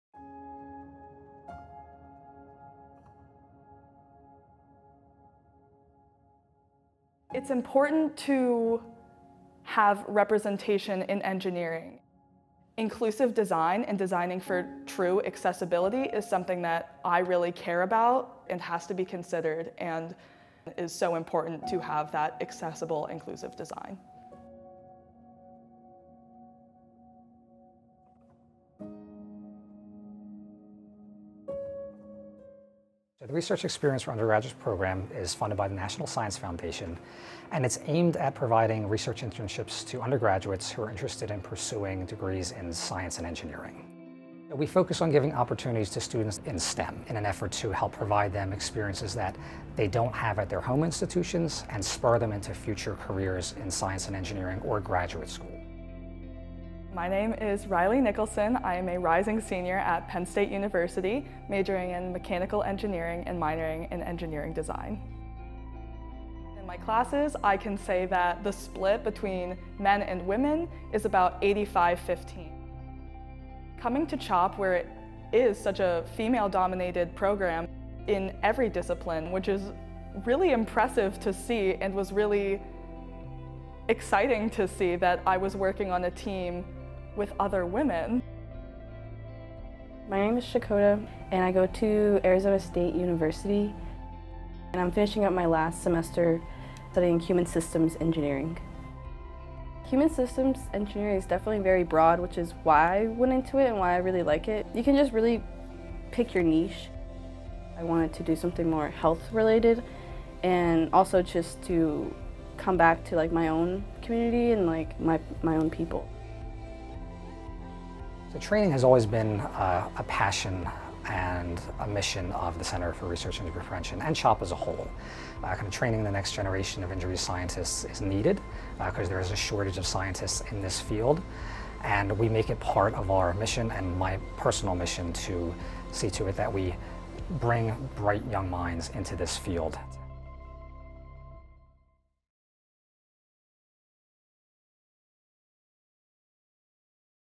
Spotlight: Injury Science Research Experiences for Undergraduates Program
The Injury Science Research Experiences for Undergraduates (REU) Program at the Children's Hospital of Philadelphia Research Institute offers students hands-on research experience and a true understanding of its impact on improving children's lives.